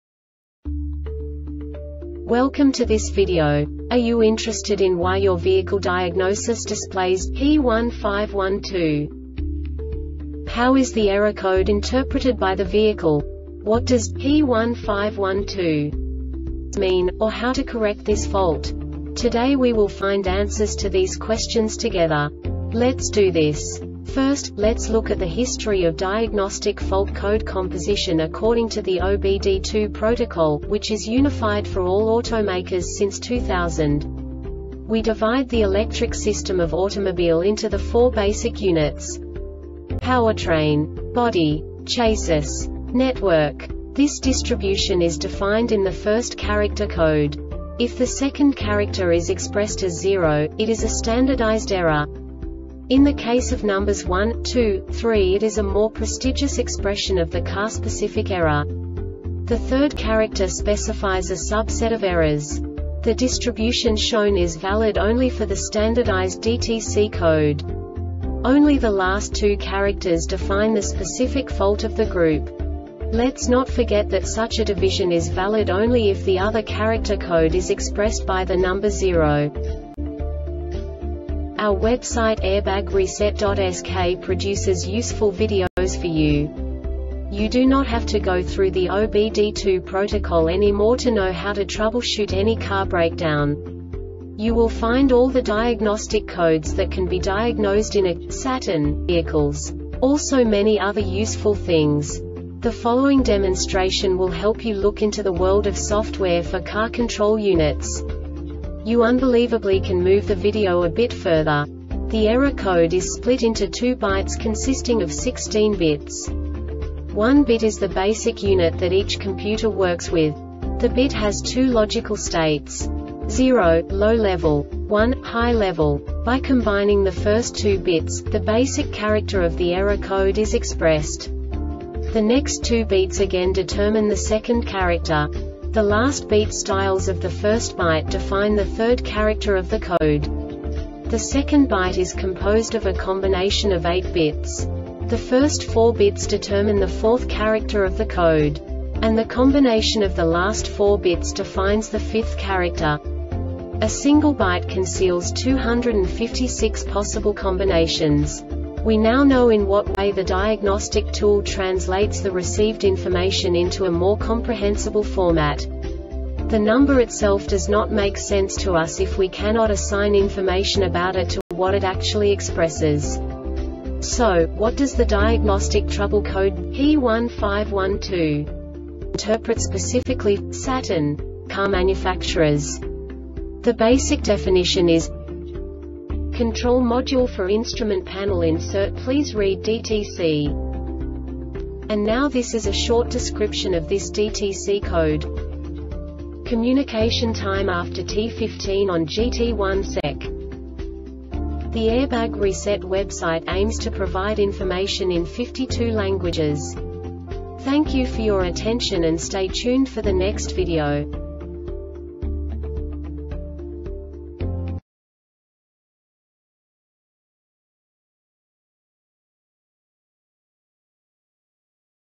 DTC Saturn P1512 Short Explanation
Contents:
0:21 Basic DTC analysis according to OBD2 protocol standard.
1:48 Insight into programming
2:58 A diagnostically understandable description of the Diagnostic Trouble Code
3:05 Basic error definition

Make:
Saturn
Code:
P1512
Definition:
Control module for instrument panel insert please read DTC
Description:
Communication time after T15 on &gt;1 Sec.